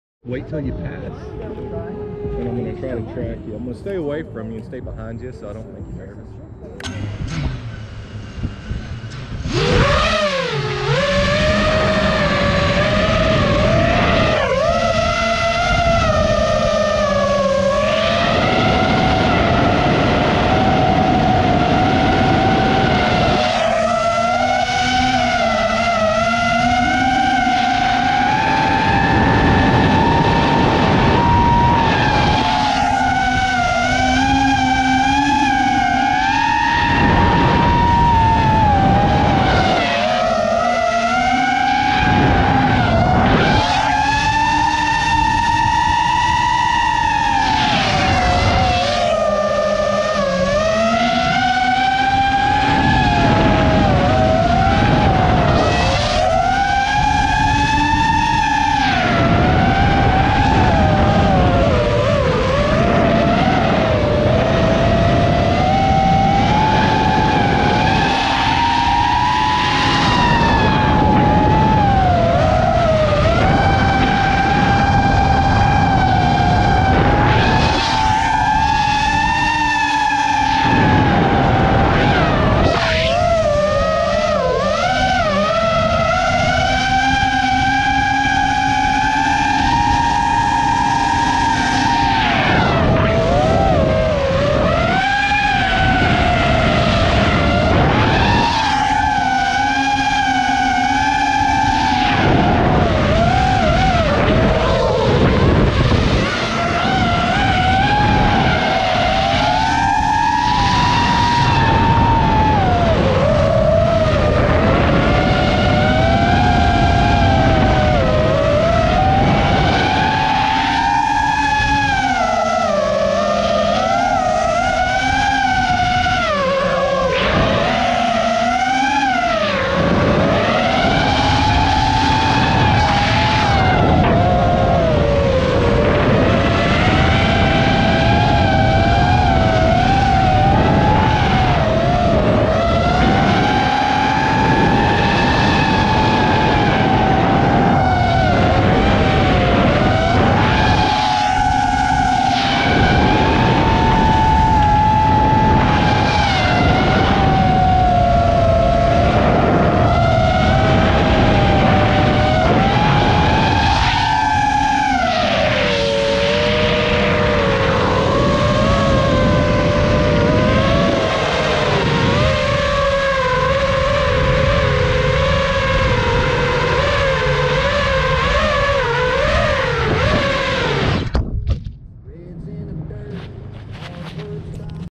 Sukhoi SU 29MM Getting Chased by quad
My Friends Mike and Anthony out flying.  Mike is flying the Eflite Sukhoi SU 29mm and Anthing is playing chase with a custom built Quad with a Hero 7 Black on it.